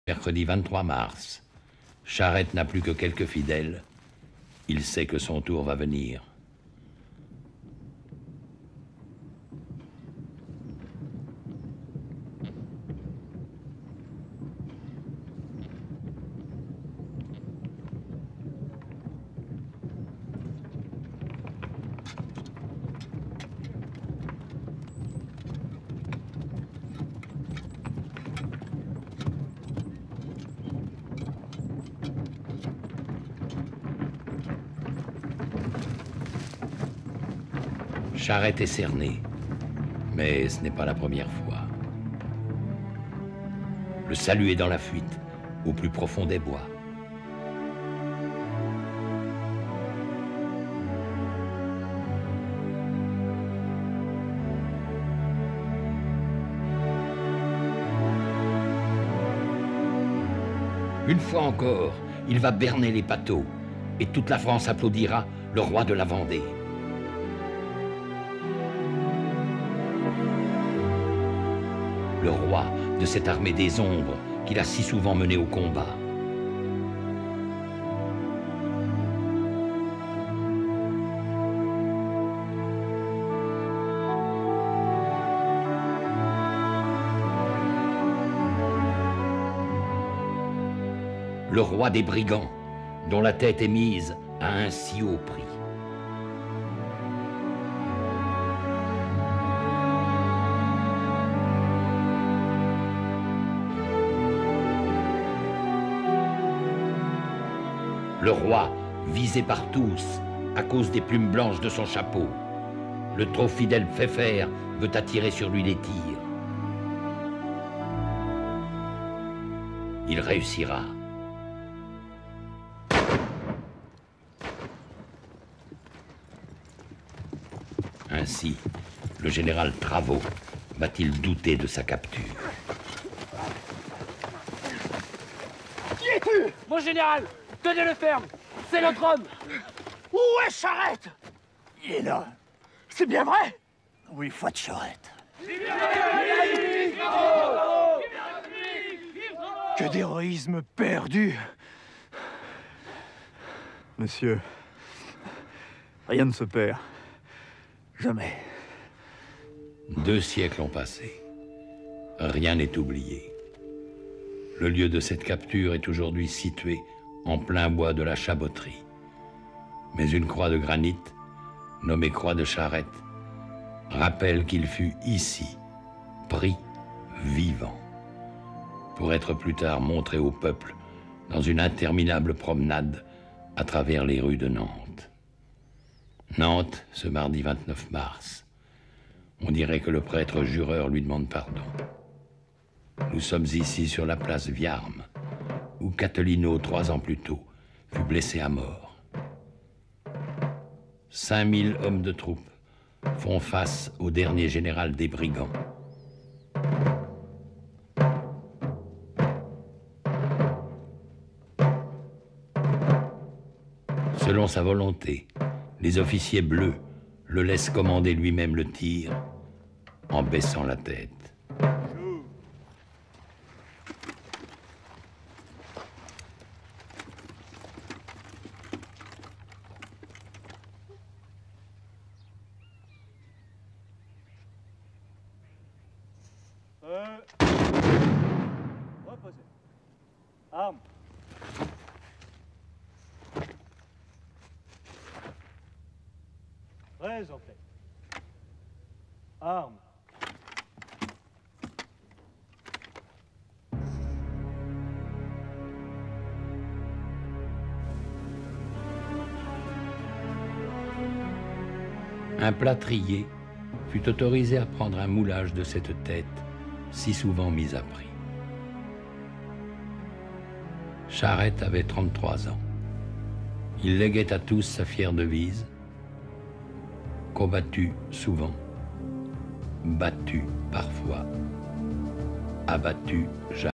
"COMBATU SOUVENT, BATU PARFOIS, ABATU JAMAIS"
"Combatido con frecuencia, batido a veces, abatido nunca"
Esta fue la divisa de Charrette, y bien podría figurar en su epitafio.
El gran héroe de la Vendee murió como vivió.
Cuando fue capturado, el gereral revolucionario, le dice "Que d'heroisme perdu!" y Charrette responde: "Monsieur, rien ne se perd, Jamais!". ("¡Cuanto heroismo perdido!" - "Señor, nada se pierde, ¡Jamás!") Nobles palabras que equivalen a un verdadero acto de Fe.